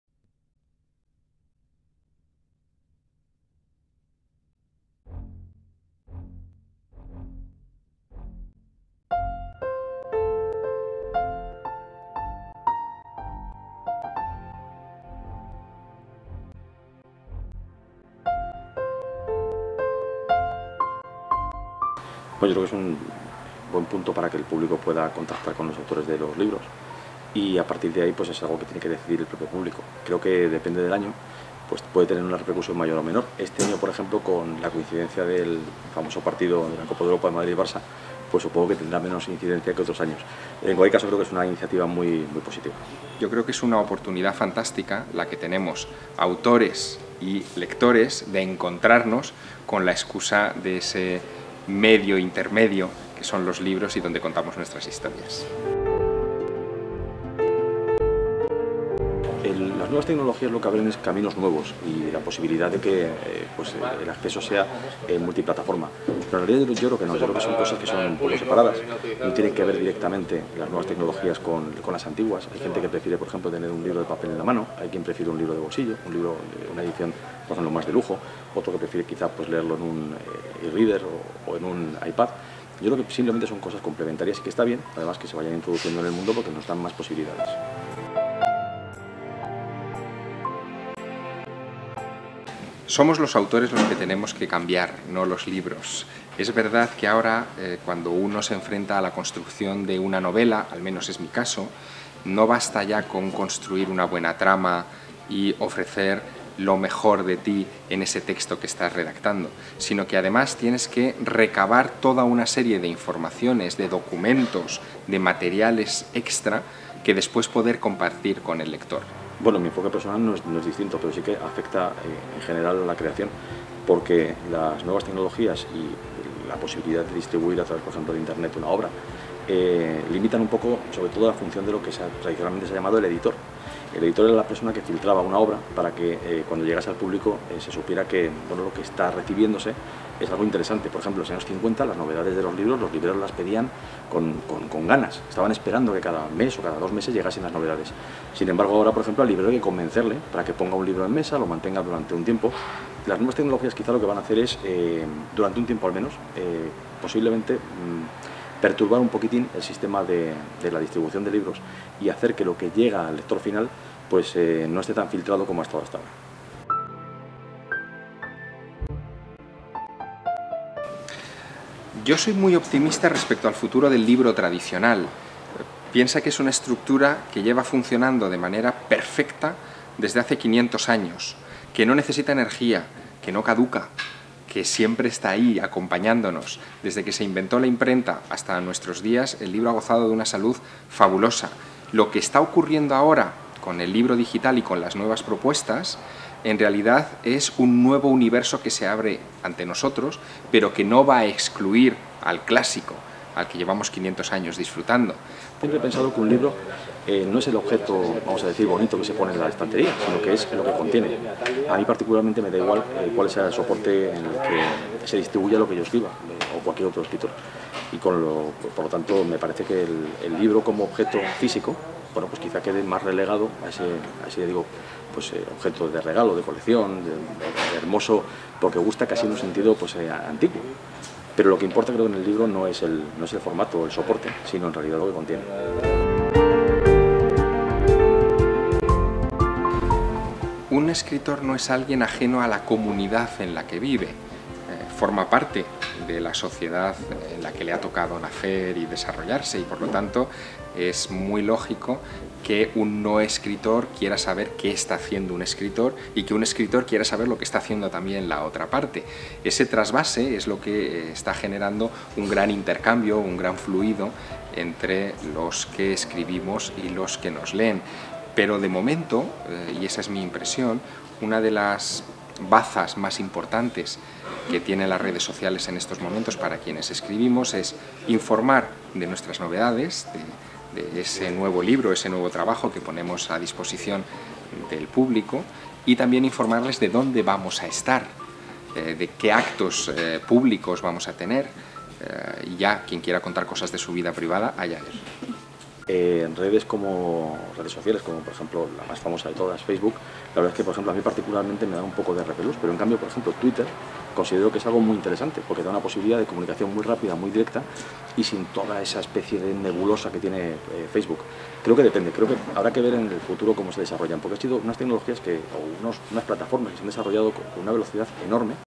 La Noche de los Libros en CAMON Madrid
Javier Sierra y David Zurdo fueron los protagonistas de La Noche de los Libros en CAMON Madrid.